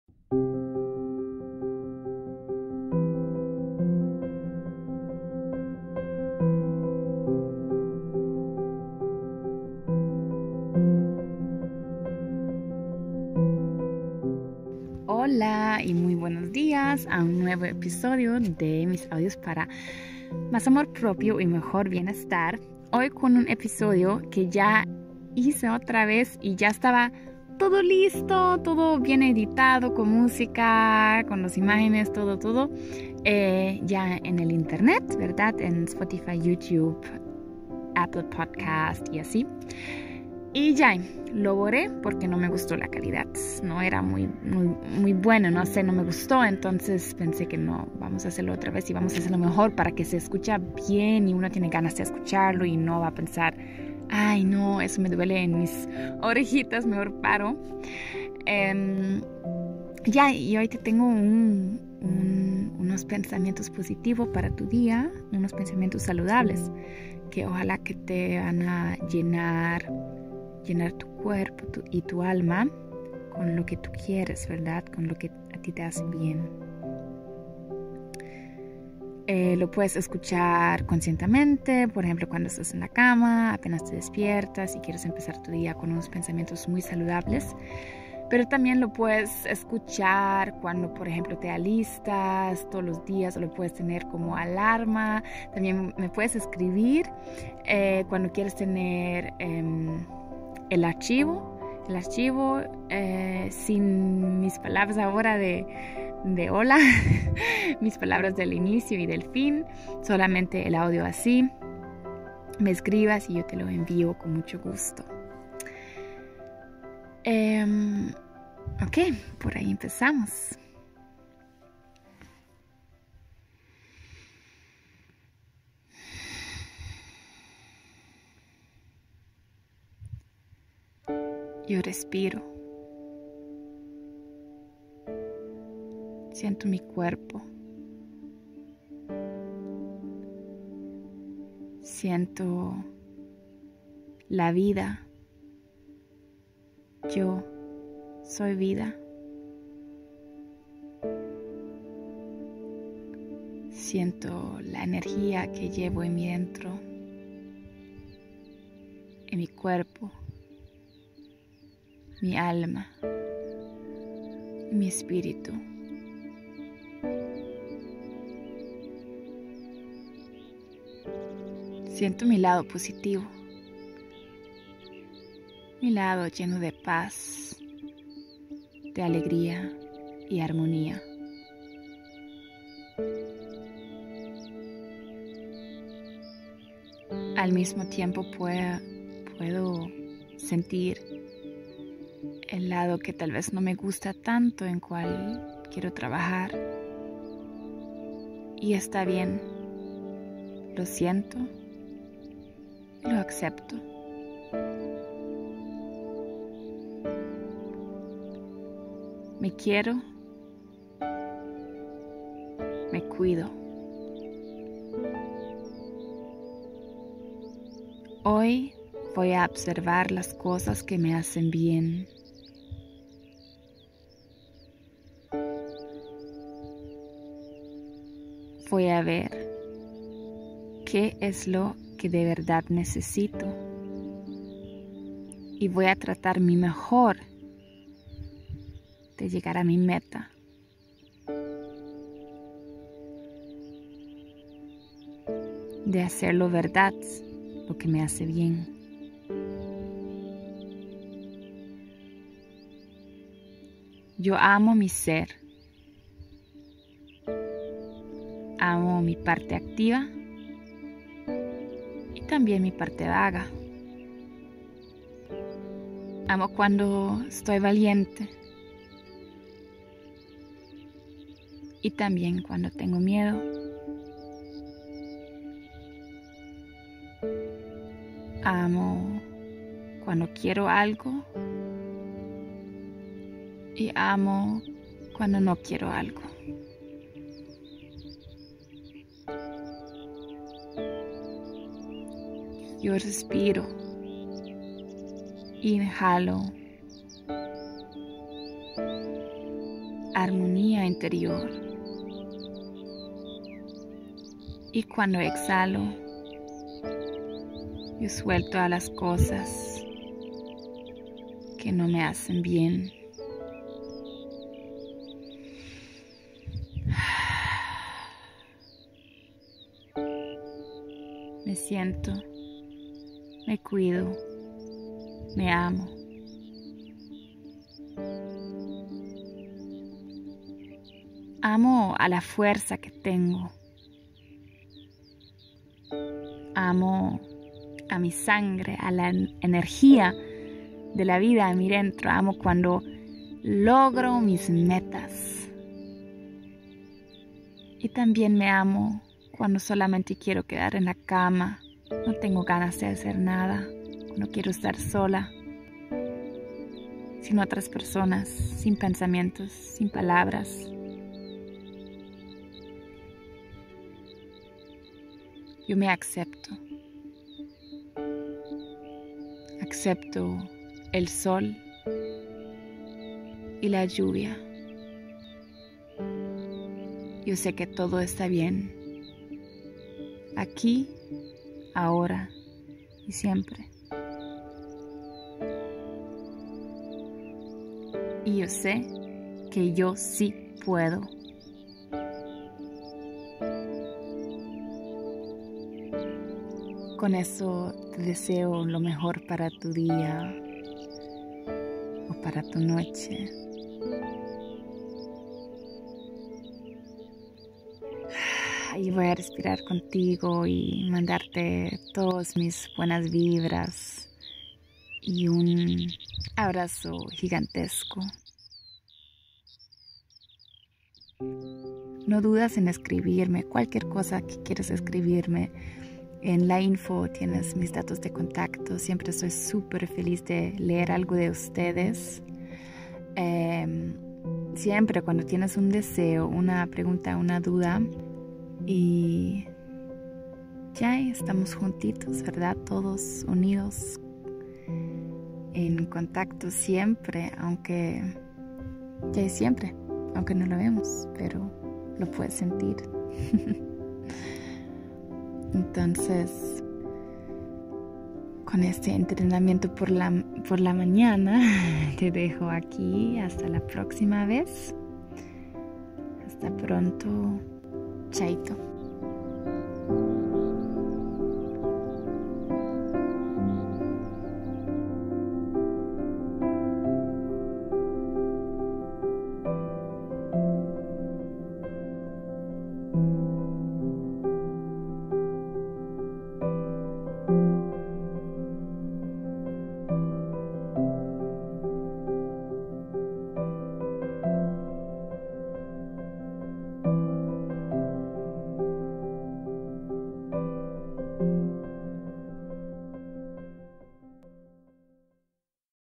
Pensamientos saludables para tu vida cotidiana: Un momento de calma para ti
El entrenamiento mental, la pelota de energía mental o como lo llaman en íngles "Energyball", está de vuelta con un resultado largamente esperado: afirmaciones positivas número 2.
¡Date un momento de sentir, de relajación, un momento solo para ti!
Diviértete mucho con eso. ♥ ️

Con cariño Jennifer

Ahora tambien encuentras el podcast en iTunes/ ApplePodcasts.